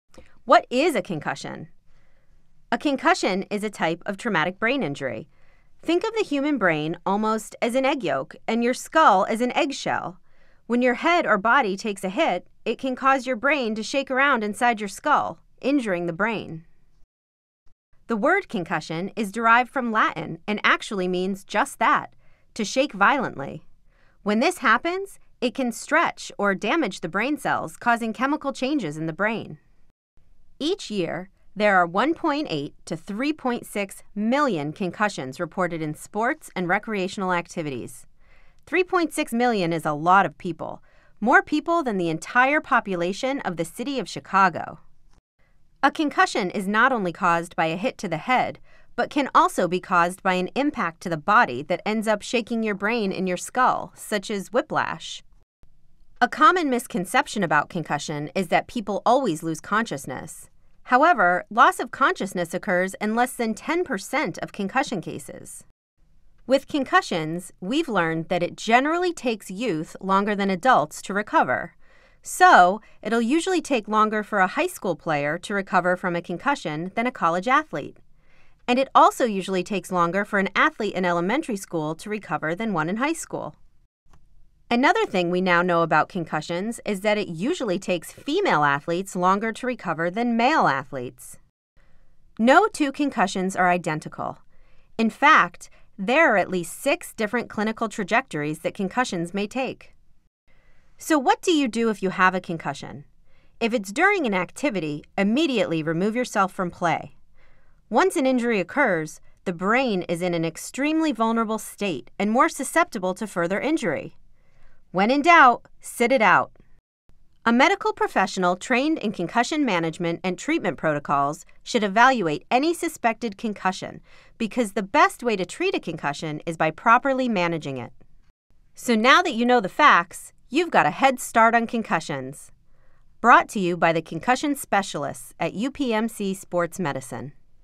What is a Concussion? | UPMC Sports Medicine Concussion Program
Learn more about the basics of concussions, including where the word comes from, common misconceptions, and what to do if you think someone might have a concussion.